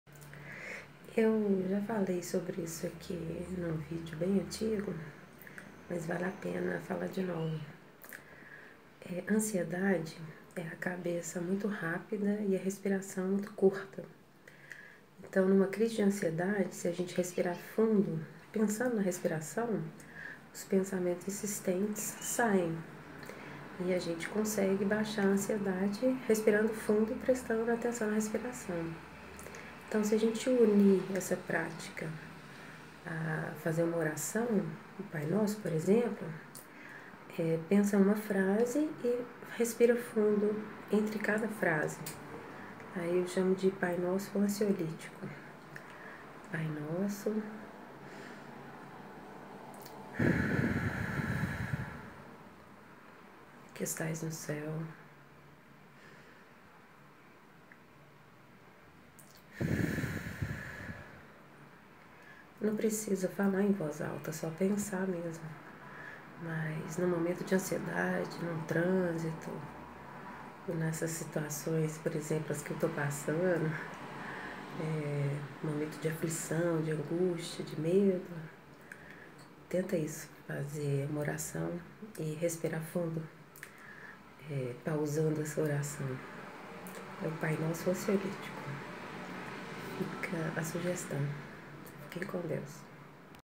Ansiedade
Respirar fundo , pensando na respiração e deixando os pensamentos negativos insistentes saírem de cena... como nuvens negras que se afastam deixando o céu mais limpo... nos tira de uma crise de ansiedade 😌